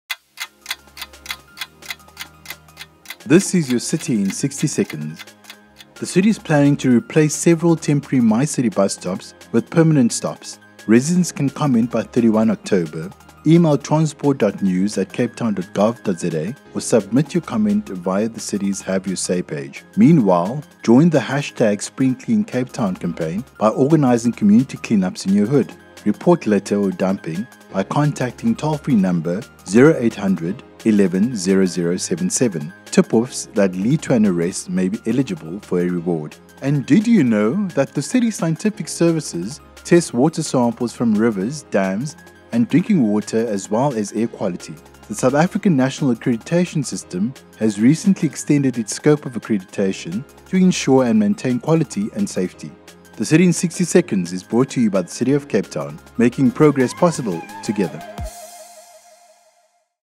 Your City in 60 seconods - 13 October 2023
With so much news these days, it’s easy to miss out on things that impact you and your community.

• The City is planning to replace several temporary MyCiTi bus stops with permanent stops
• Join the SpringCleanCT campaign by organising community clean-ups in your hood.
• City’s Scientific Services tests water samples from rivers, dams and drinking water as well as air quality.

In 60 seconds, get an overview of what’s going on and opportunities in your City right now!